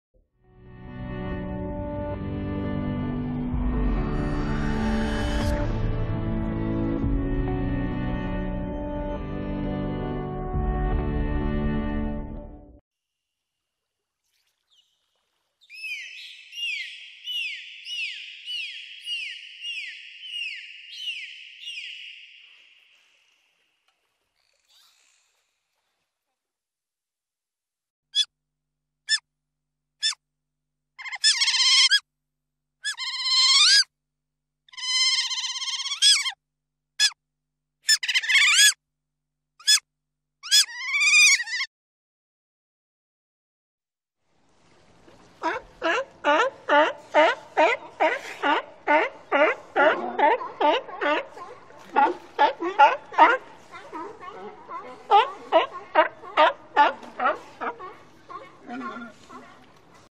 Dolphins/Sea Lion/Otter, Vocalizing
Dolphins Vocalizing:   Duration: 15.07s
                                         Bitrate: 192kbps
                                         Sample Rate: 44khz

Otter Vocalizing and Chirping :  Duration: 15.75s
                                                           Bitrate: 192kbps
                                                           Sample Rate: 44khz

Sea Lion Barking : Duration: 54.94s
                                  Bitrate: 192kbps
                                  Sample Rate: 44khz